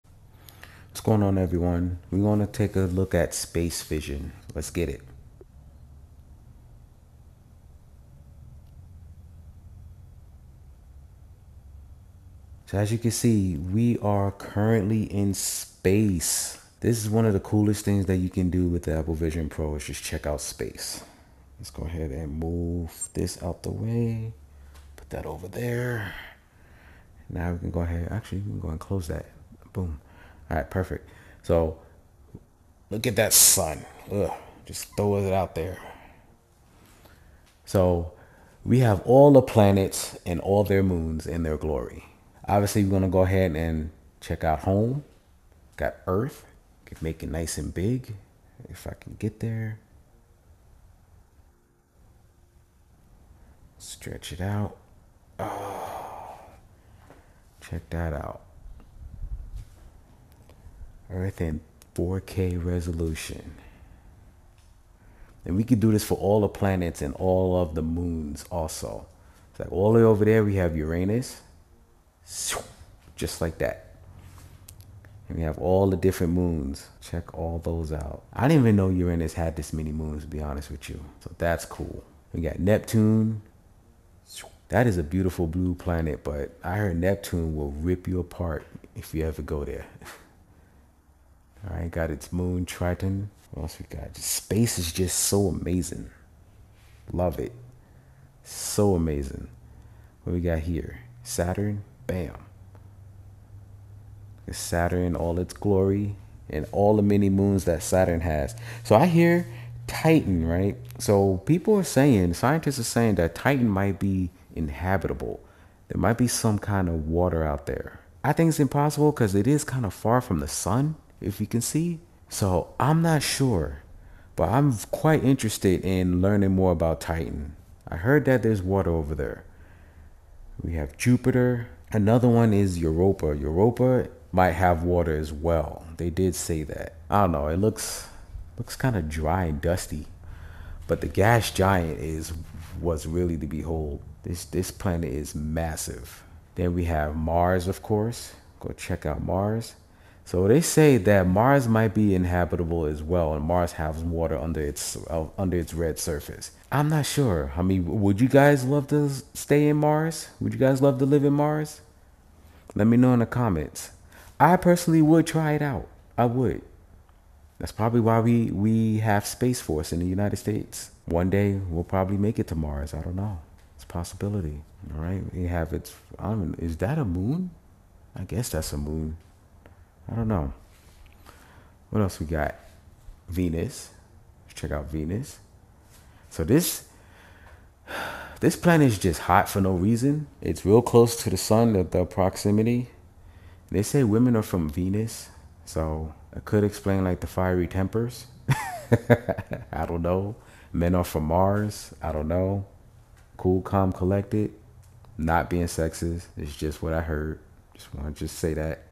I Explored Space With Apple Vision Pro And It's Insane
🚀 What if you could explore the vastness of space… without leaving your home? 🌌

In this video, I dive into the universe like never before using the Apple Vision Pro. With its immersive VR environments and stunning visuals, the Vision Pro takes us on a journey through galaxies, nebulae, and the infinite stars above.

You’ll experience:
✨ The feeling of floating in zero gravity
🌍 A breathtaking view of Earth from orbit
🌌 Flying past distant planets, moons, and galaxies
🛰️ How the Apple Vision Pro transforms VR into a real space experience

This isn’t just a simulation — it’s a mind-blowing journey through the cosmos, powered by the future of spatial computing.

👉 Would you travel to space if you had the chance? Drop your answer in the comments!